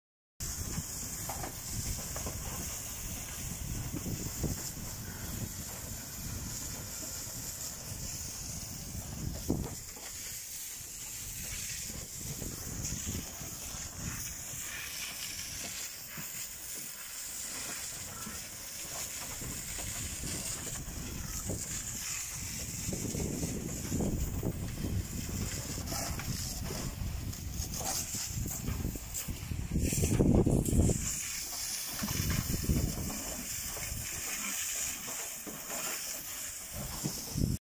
Así es como viajan los cerdos para antes llegar ala mesa navideña.
Una previa refrescada antes de viajar al camal.